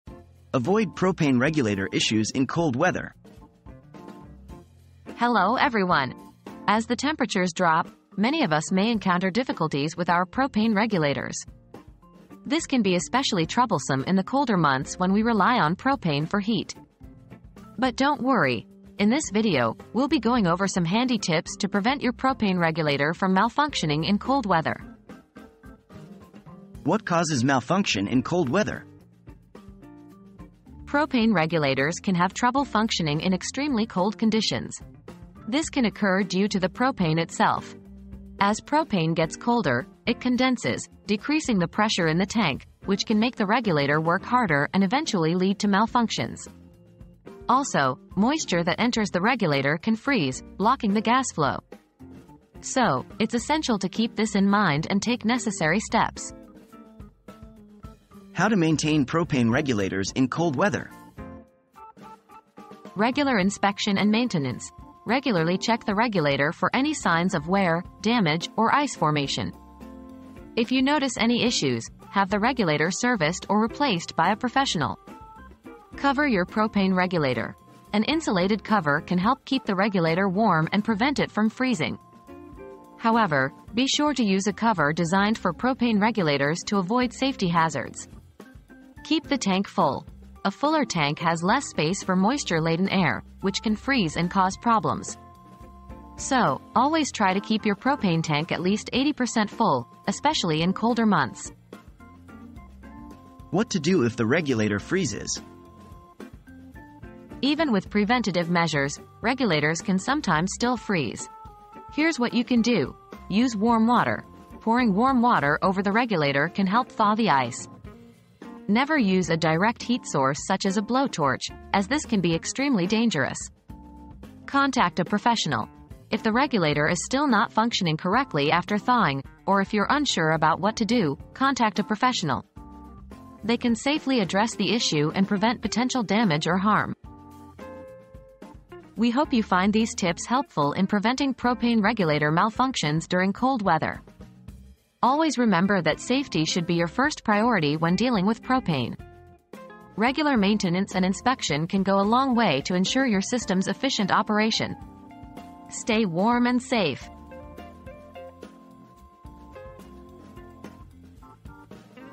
Avoid Propane Regulator Issues in Cold Weather
How can I prevent my propane regulator from malfunctioning in cold weather? • Learn how to prevent your propane regulator from malfunctioning in cold weather with these helpful tips. In this video, we'll explain the causes of regulator malfunction, provide maintenance tips, and guide you on what to do if your regulator freezes. Don't let the cold weather disrupt your propane supply - watch now to keep your regulator working smoothly!

00:00 • Avoid Propane Regulator Issues in Cold Weather
00:26 • What Causes Malfunction in Cold Weather?
00:58 • How to Maintain Propane Regulators in Cold Weather?
01:43 • What to Do if The Regulator Freezes?